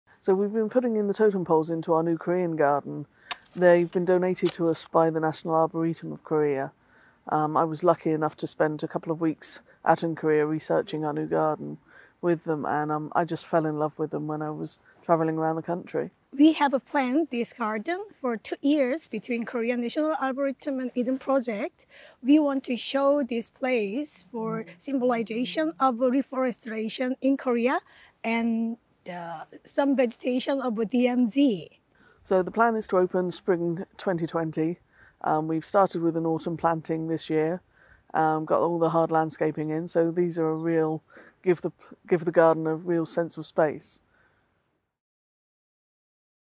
Korean Garden to open at the Eden Project in 2020
The new Eden garden, which will be the UK's first Korean garden, is themed around the unique plants of the Korean peninsula and the extraordinary reforesting of South Korea over the last 50 years. The garden occupies a 1,000 square metre space on the top edge of the Eden outdoor gardens close to the Visitor Centre.

The garden will open in spring 2020.